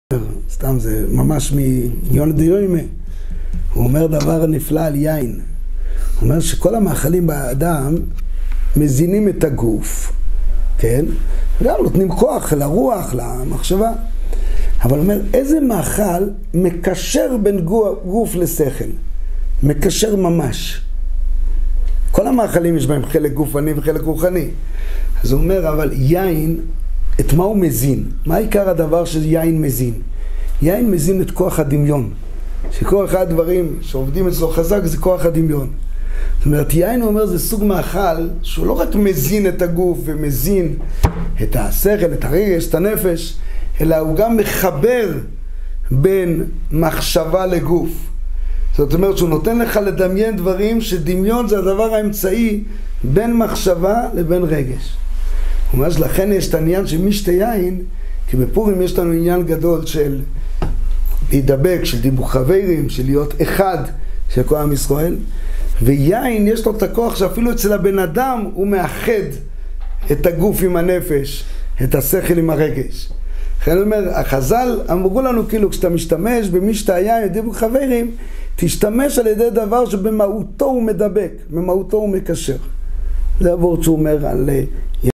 Le vin et l'imaginaire -הרב הוטנר על הקשר בין יין לדמיון
טעם למשתה היין בפורים , הקשר בין יין לדמיון - הרב הוטנר
Raison pour laquelle on a choisi le vin pour le festin de Pourim, le lien entre le vin et l'imagination - Rav Hutner

קטע מוועד 2 חלק 2 : 08:24 - 10:40
 Extrait du Cours n°2 partie 2: 08:24 - 10:40